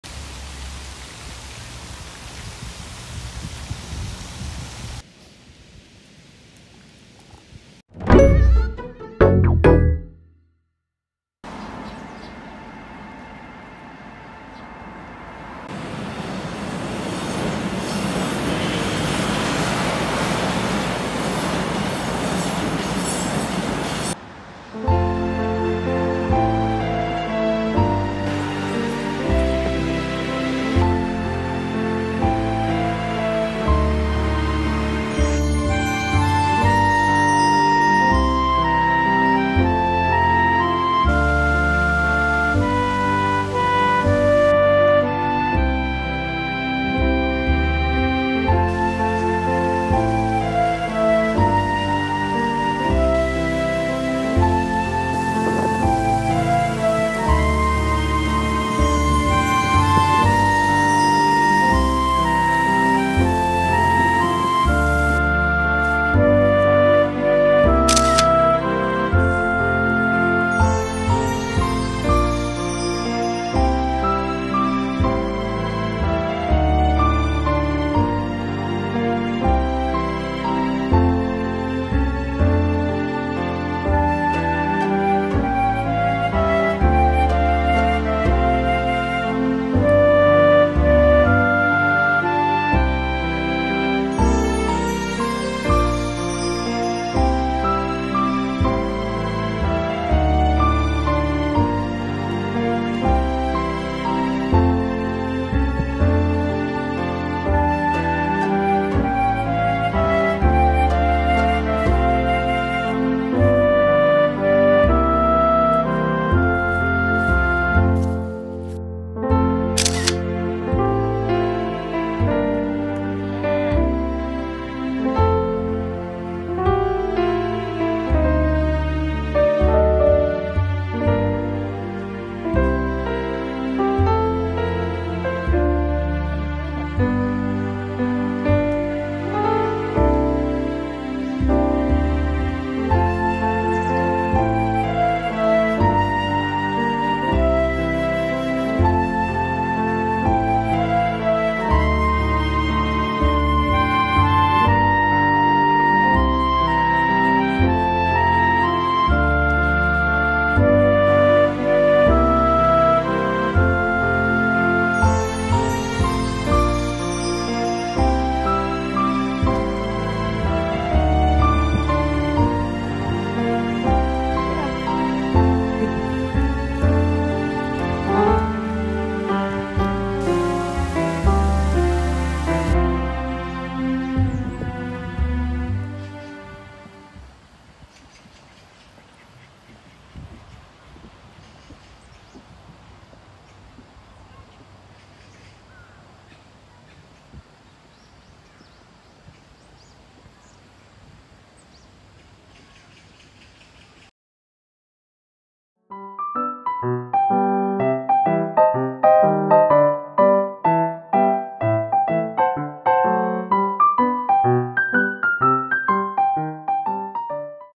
냄새 킁킁☘️ 강아지와 용산가족공원 산책하는 날☘️Dog Walking🍁Pug Kongzi 🍂Yongsan Family Park🪷
🐶콩쥐랑 용산가족공원 산책 왔어요.
바람 솔솔🌤
행복한 가을 맞이하세요🌾

이름 : 콩쥐
성별 : 여아
나이 : 6살